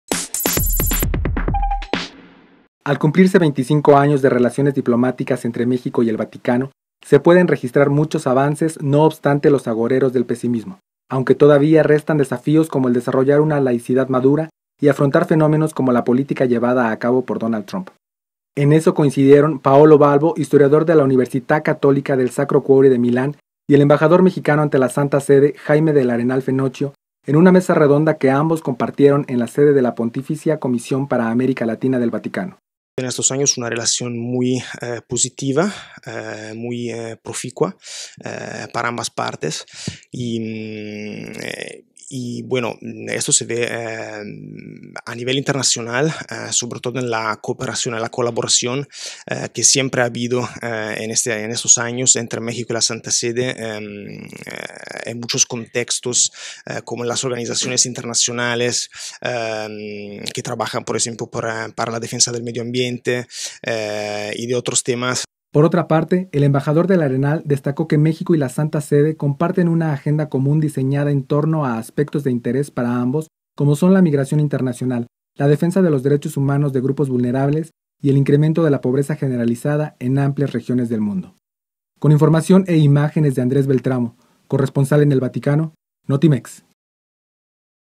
Avances y desafíos marcan 25 años de relación México Vaticano
En una mesa redonda en la sede de la Pontificia Comisión para América Latina del Vaticano, se discutieron temas sobre los 25 años de la relación México-Vaticano